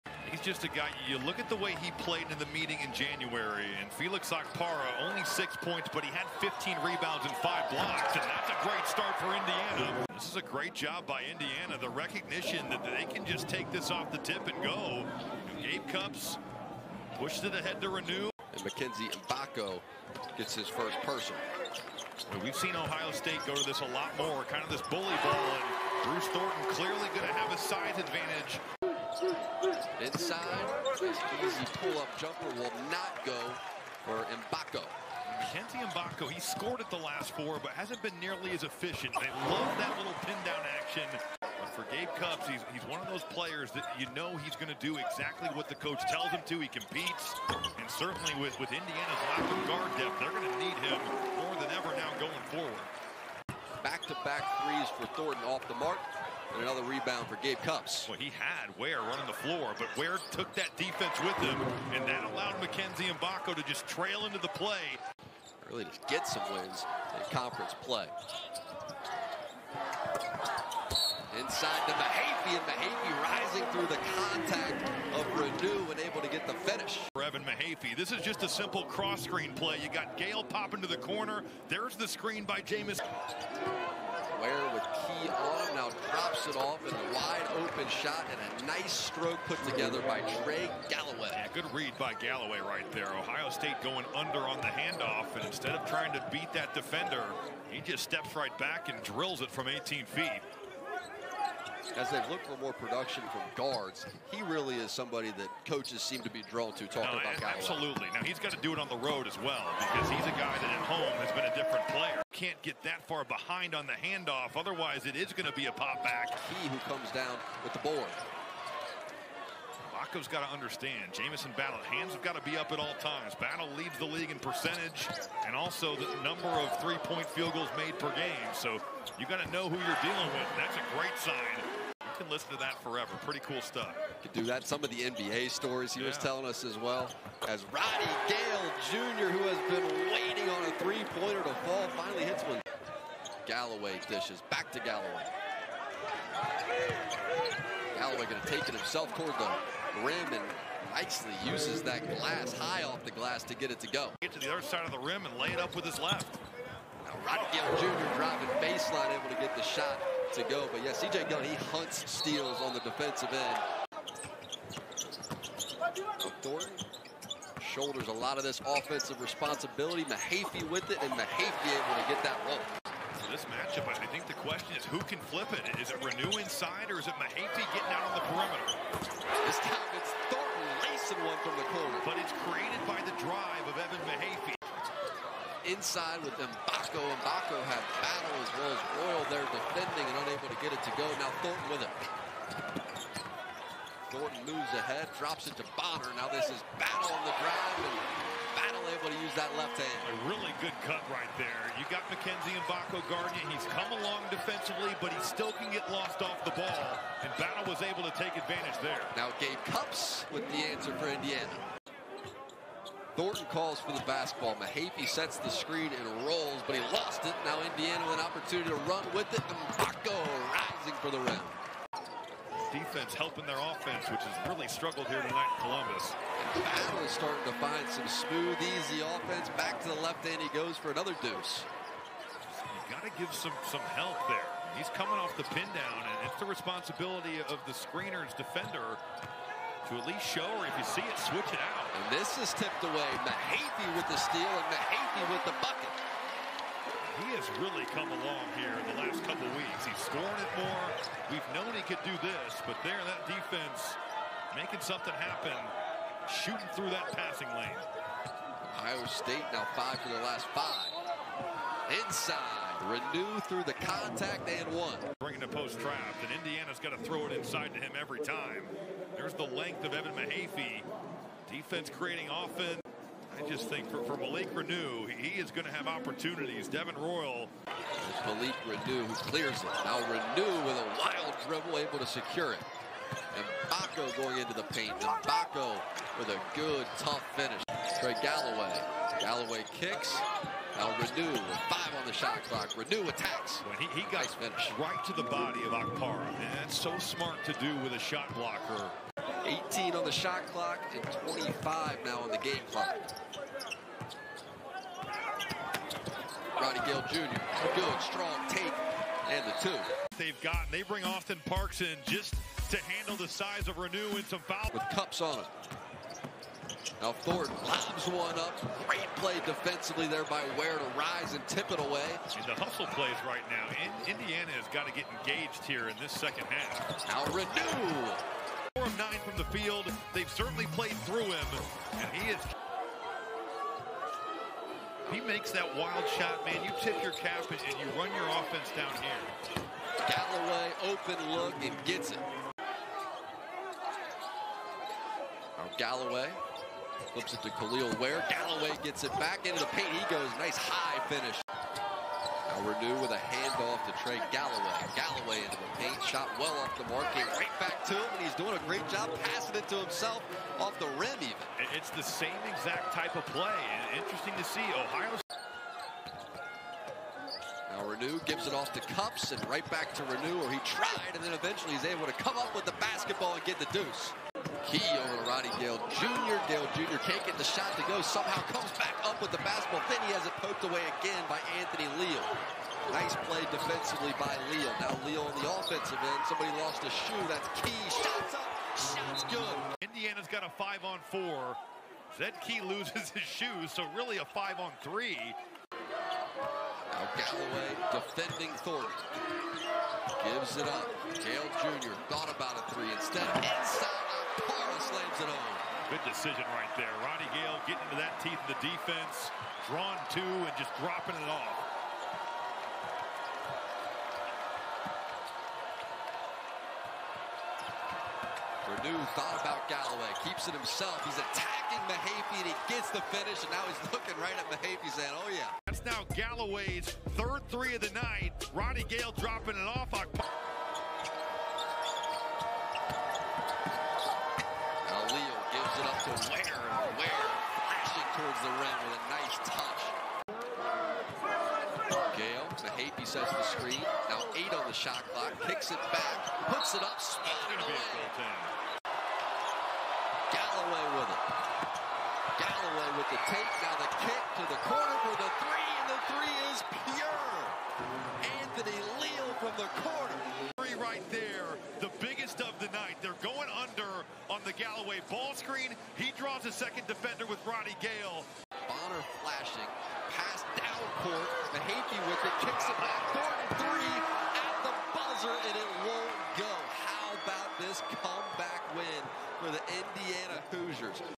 Indiana at Ohio State | Highlights | Big Ten Men's Basketball | Feb. 6, 2024
00:00:00 - 1st Half
00:05:13 - 2nd Half